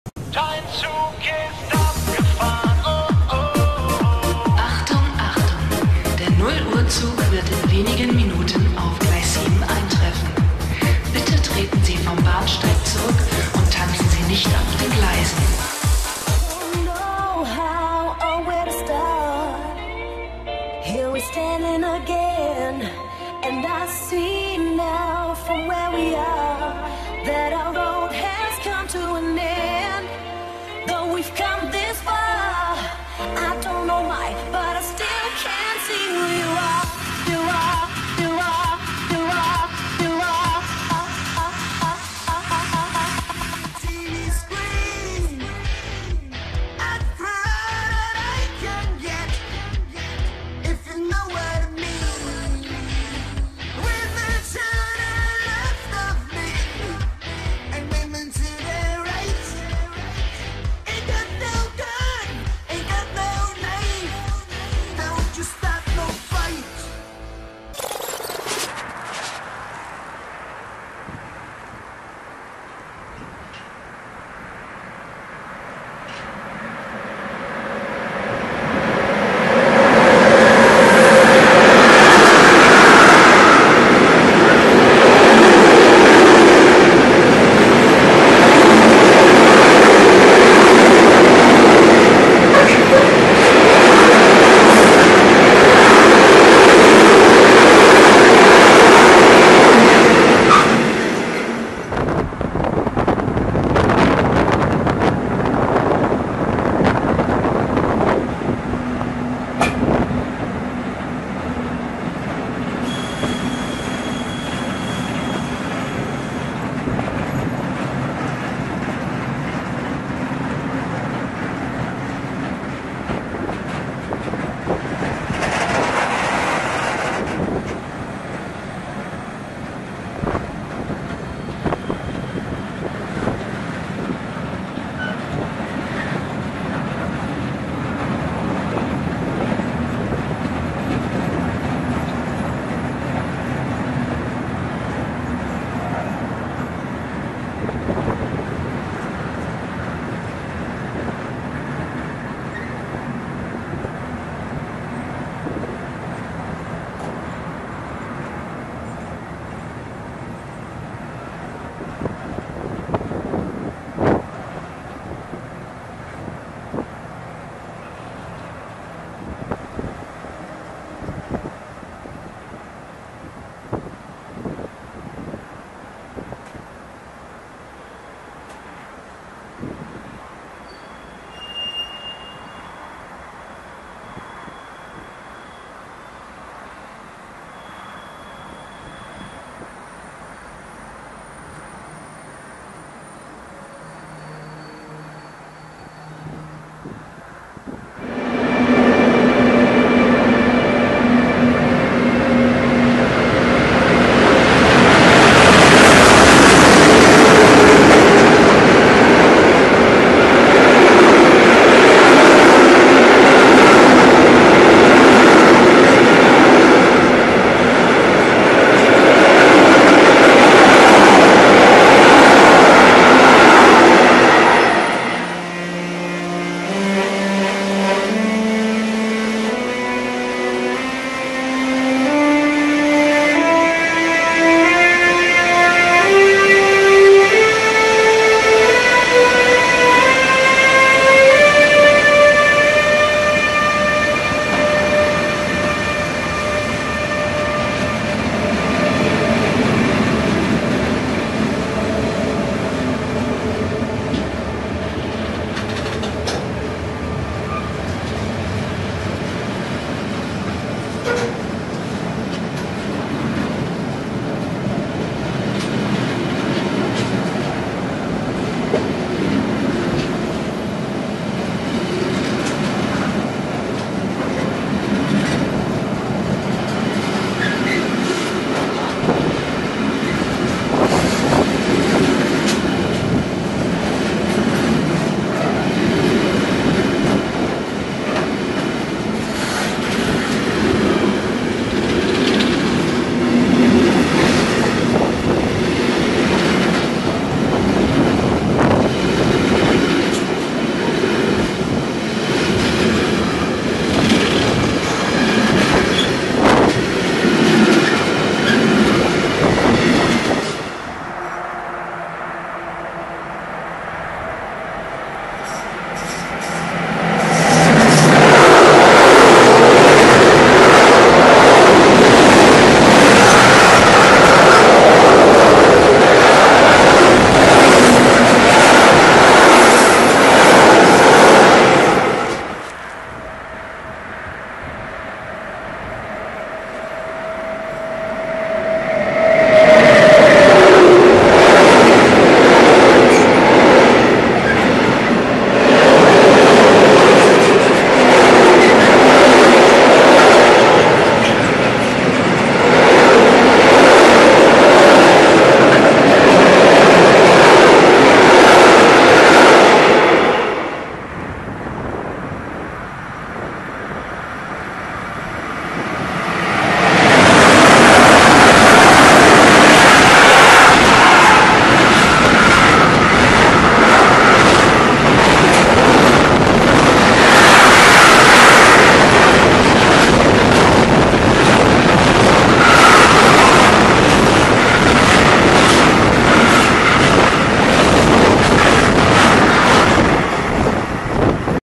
Bahnverkehr in Lupfig am 10.02.12 - ES 64 U2 ganz nah!!!
Bahnverkehr in Lupfig am 10.02.12 mit SBB, Crossrail und TXL!